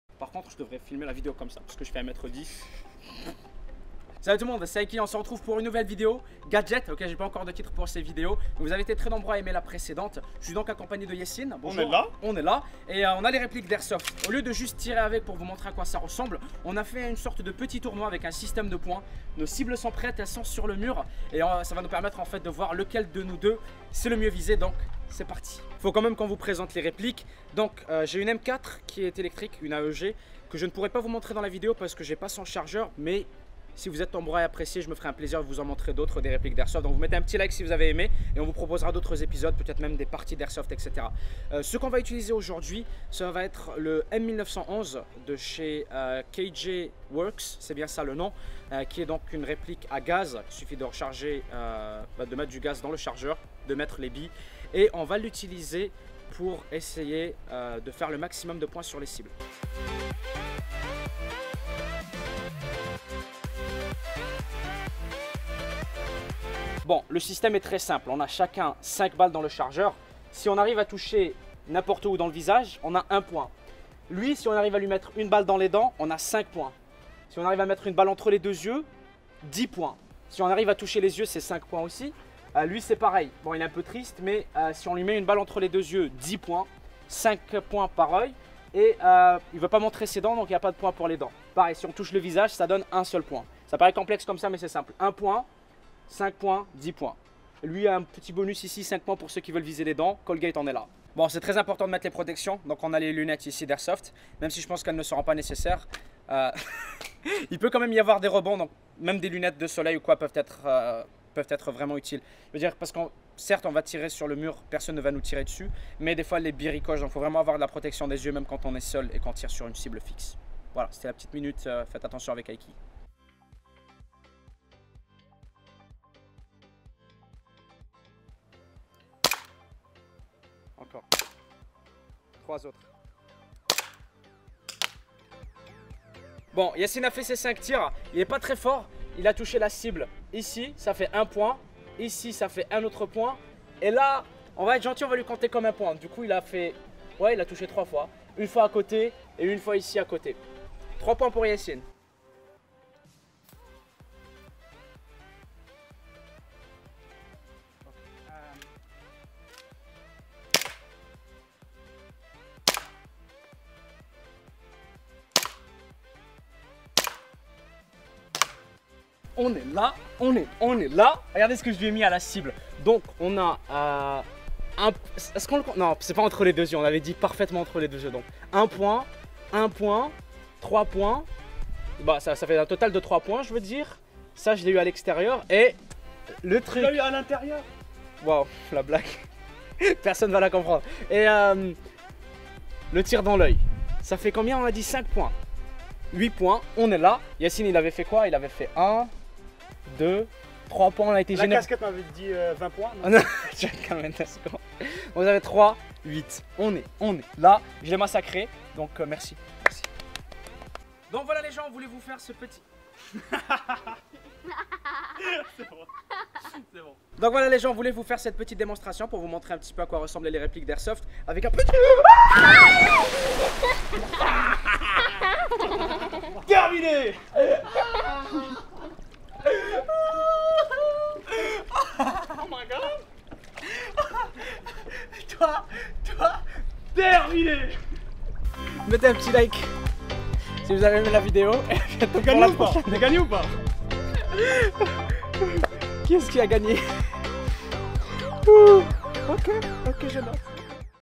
Aietech #2: Concours de tir près de la piscine
Bon, vous avez été super nombreux à like la précédente vidéo et à demander les répliques d'airsoft, en voici donc une petite vidéo! Go nous follow sur Instagram:

La vidéo est un montage, les scènes ne sont donc pas toutes forcement des suites les unes des autres, des fois c'est de la mise en scène, donc n'oubliez pas, portez vos lunettes d'airsoft en tout temps :)

Répliques :
KJW M1911
Vega Force Company M4
Billes: 0.25g